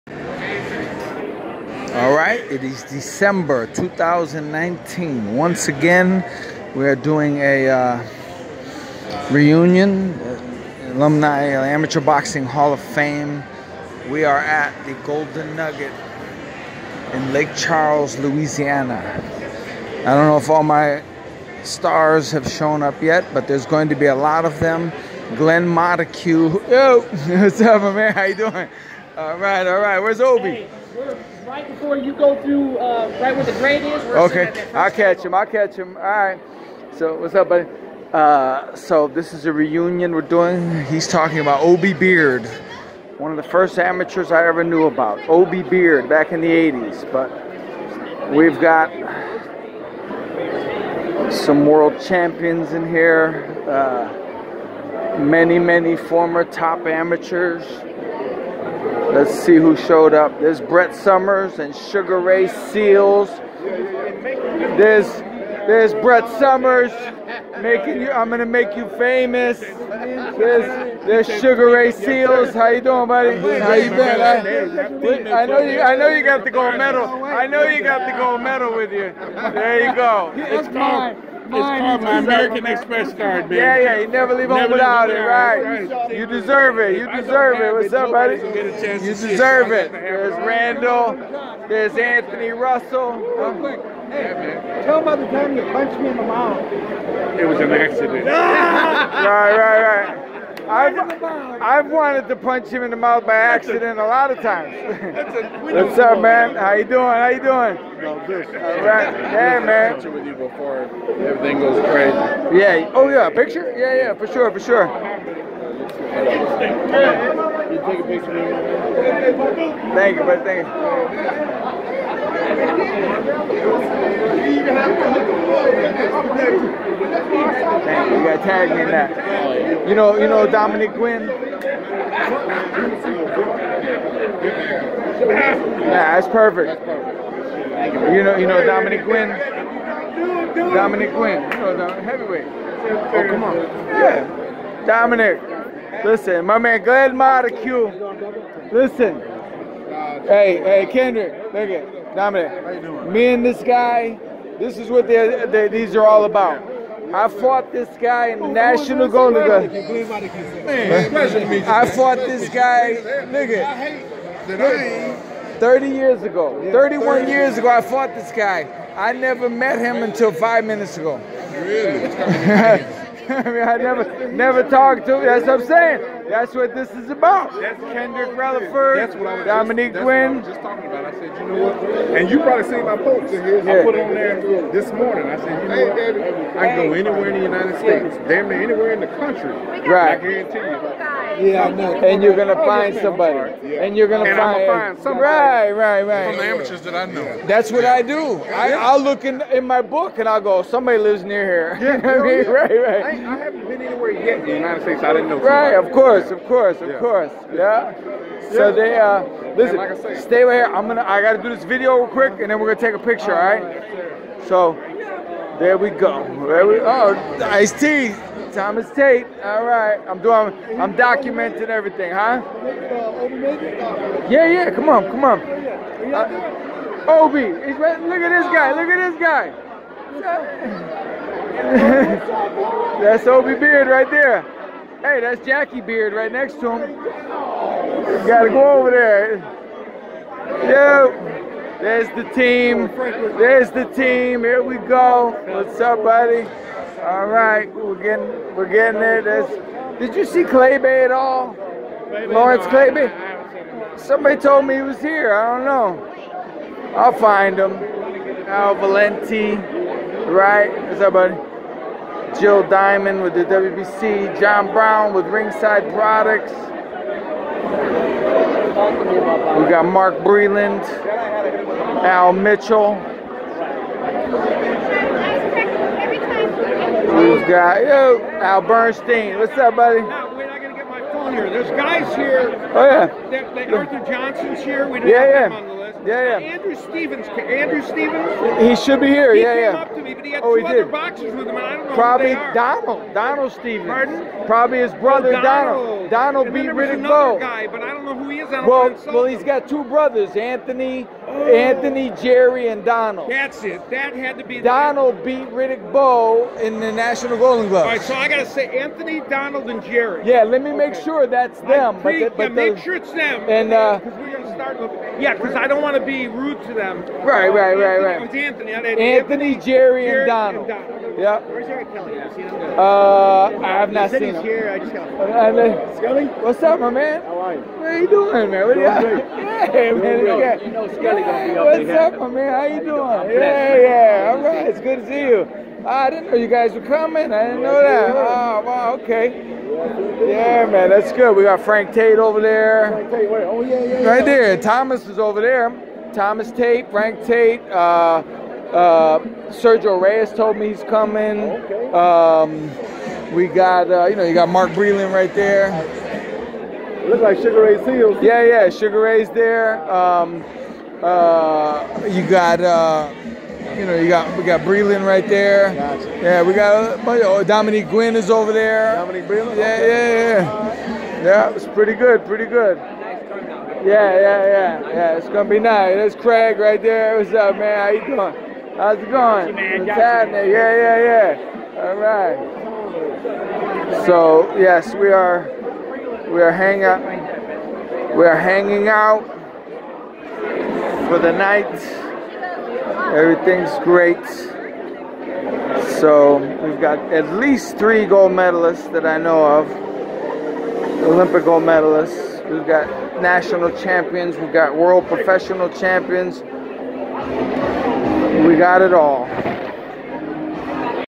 Historic USABoxing Amateur Reunion, December 2019 @ Lake Charles, Lou.
A special night in Louisiana! Many many friendships rekindled